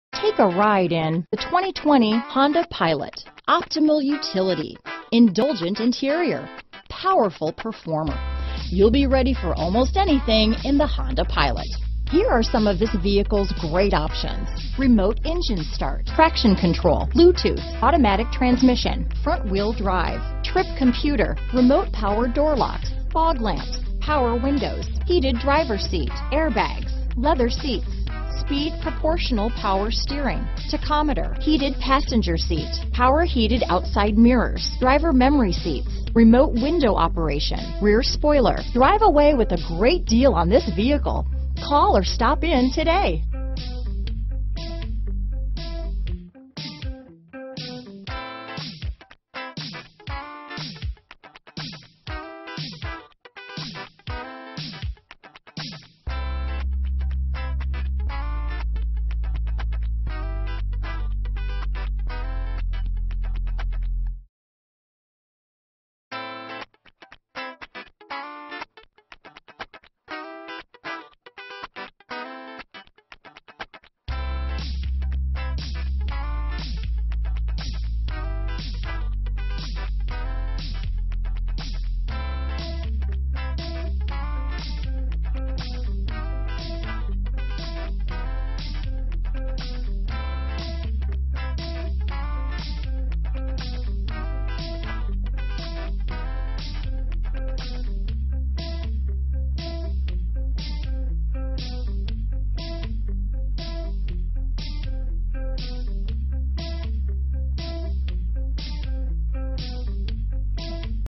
2020 Honda Pilot Homestead, Miami, Kendall, Hialeah, South Dade, FL 61816
Black Forest Pearl New 2020 Honda Pilot available in Homestead, Florida at Largo Honda. Servicing the Miami, Kendall, Hialeah, South Dade, FL area.

Largo Honda
554 NE 1st Ave

Need gas? I don't think so. At least not very much! 27 MPG Hwy.. Does it all!! This really is a great vehicle for your active lifestyle! Safety equipment includes: ABS, Traction control, Passenger Airbag, Curtain airbags, Front fog/driving lights...Other features include: Leather seats, Bluetooth, Power locks, Power windows, Sunroof... Largo Honda Home Delivery for Less!